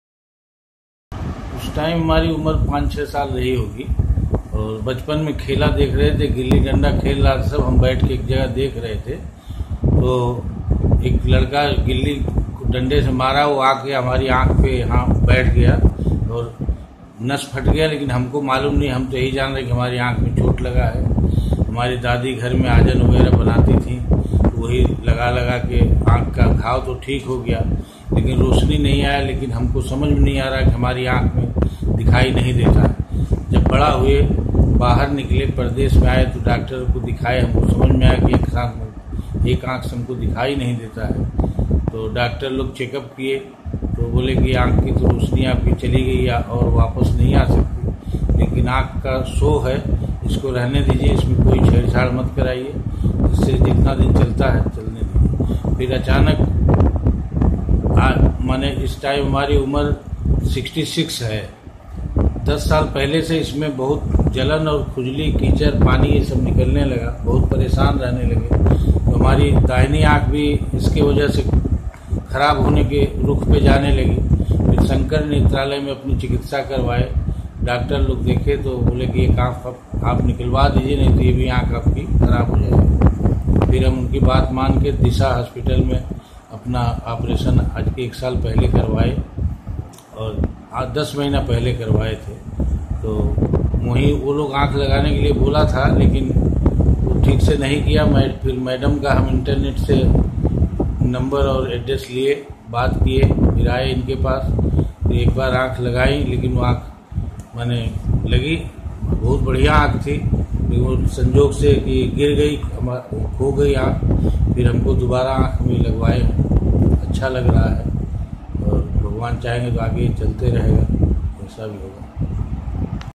Artificial eye Mr Ghana shyam story
How I lost my eye in my childhood while playing.